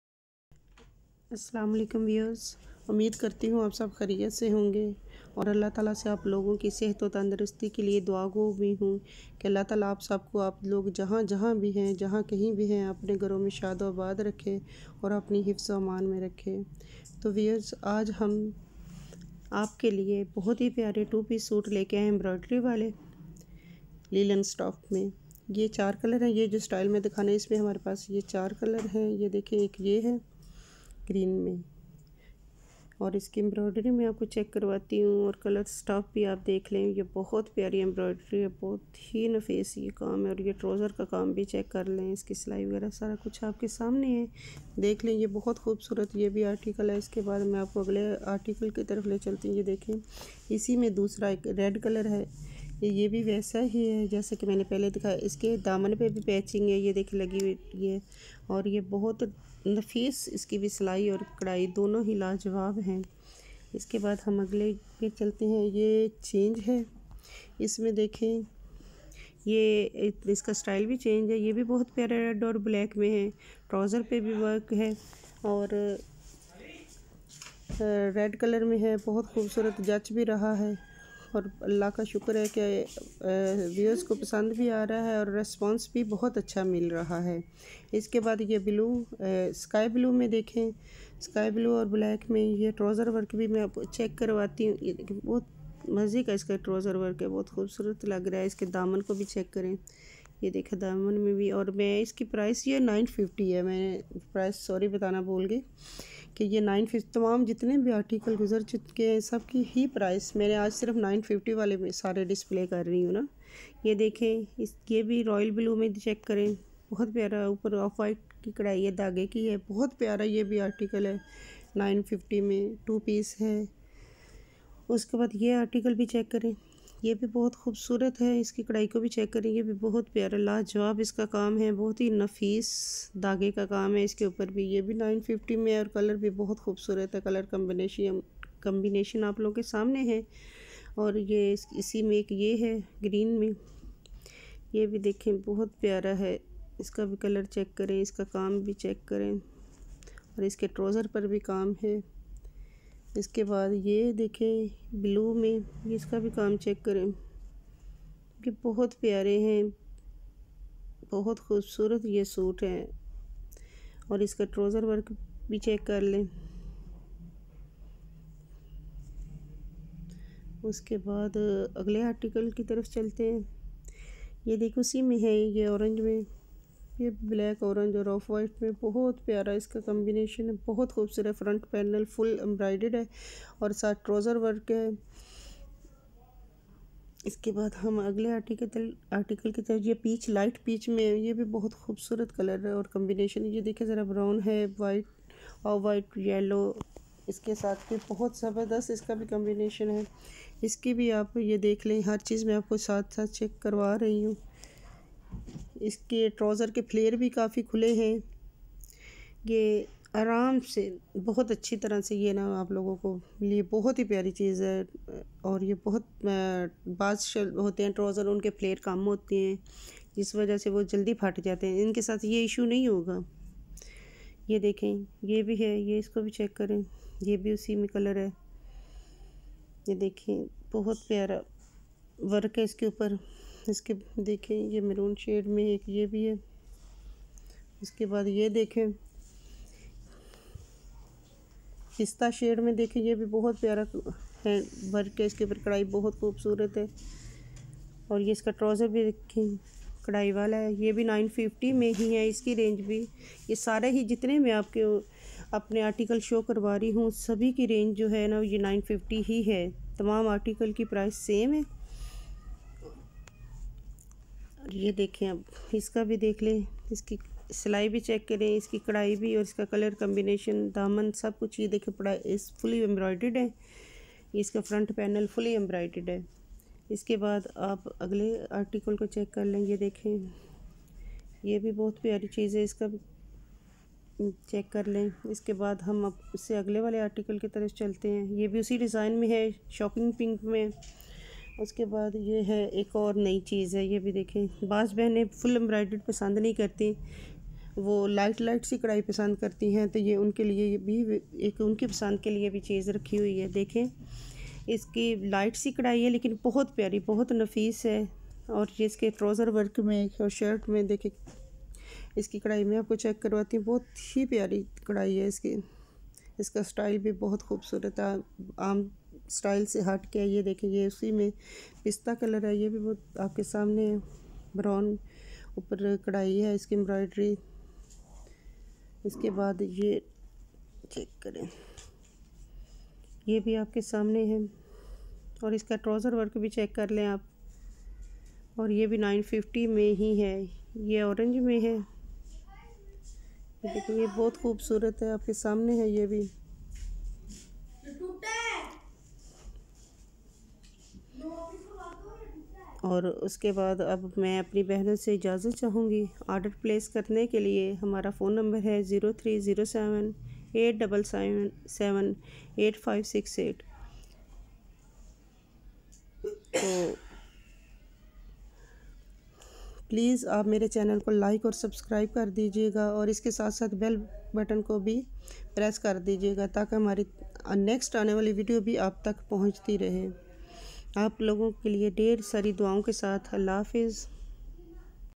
Awesome Combination of Pure Cotton | Embraided Stitched Shirts and Trousers | Sajida Gull
Two Piece embroidered suits with soft lawn -

Key Features:

- Best for Occasional, Party and Events Wears,
- Glamorous color contrast,
- Affordable Rates, start from PKR 950
- Wide designs available.

Contact/Follow us for more:

Shop Address:                 Ali Garments - Dhok Seyedan Road Dr. Zahid Wali Street Near Zohaib PCO, Rawalpindi.